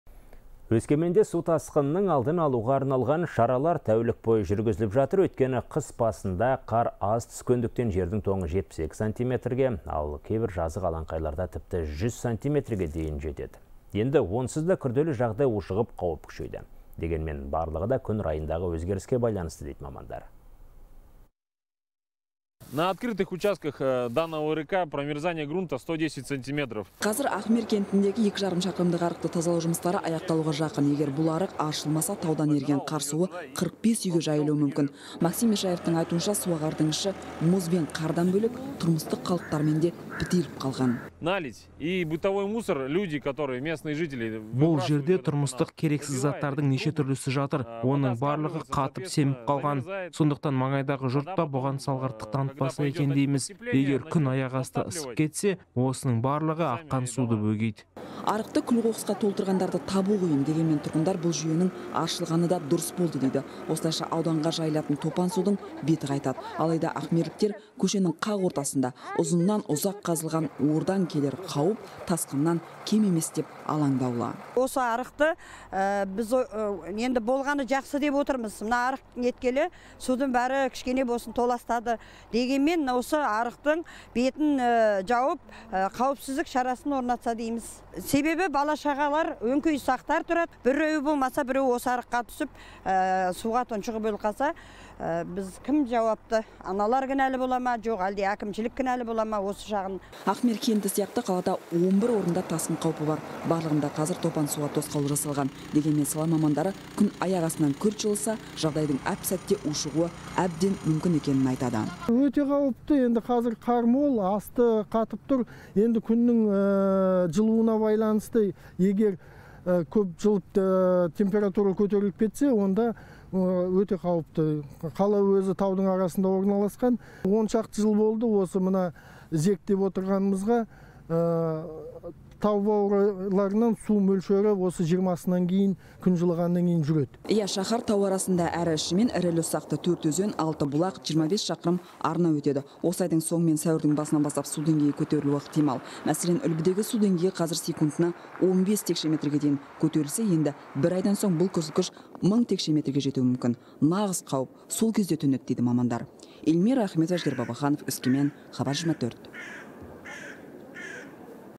Өскеменде тасқынның алдын алуға арналған шаралар тәулік бойы жүргізіліп жатыр
Өскеменде су тасқынының алдын алуға арналған шаралар тәулік бойы жүргізіліп жатыр, деп хабарлайды «Хабар 24».